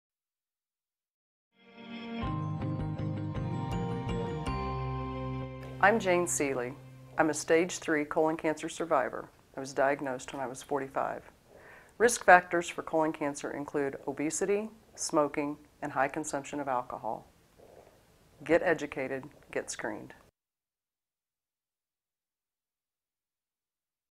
Colon Cancer Awareness 23: Jane Seely
Jane Seely | Colon Cancer Survivor - I'm a stage 3 colon cancer survivor. I was diagnosed when I was 45. Risk factors for colon cancer include obesity, smoking and high consumption of alcohol. Get educated, get screened.